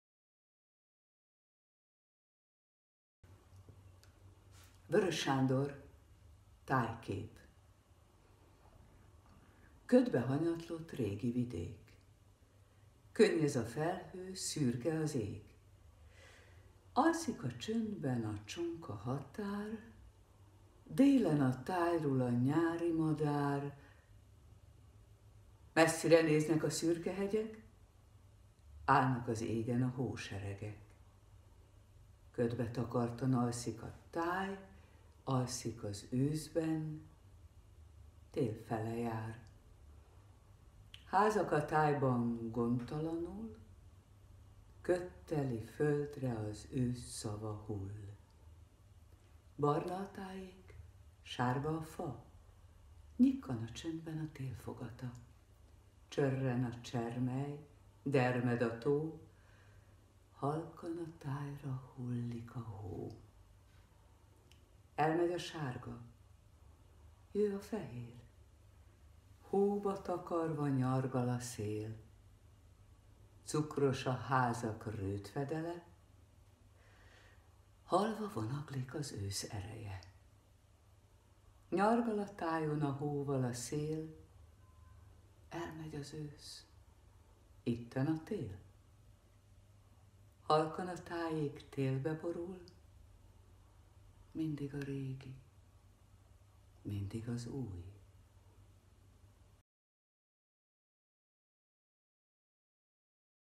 Weöres Sándor: Tájkép
Elmondja: Takács Katalin